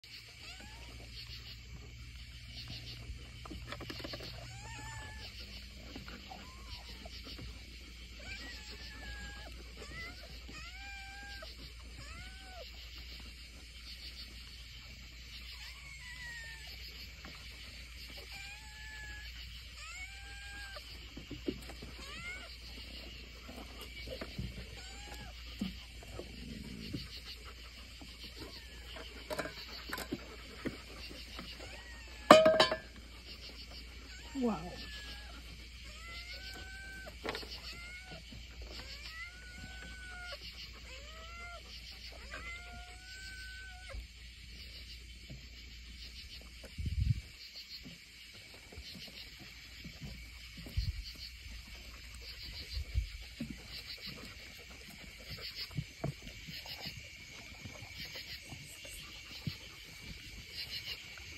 Saturday night rendezvous with furry friends 🤣
Fed one furry 🐈‍⬛friend last night now the entire pet 🐾🦮community is after me🏃🏽‍♀️😰🤣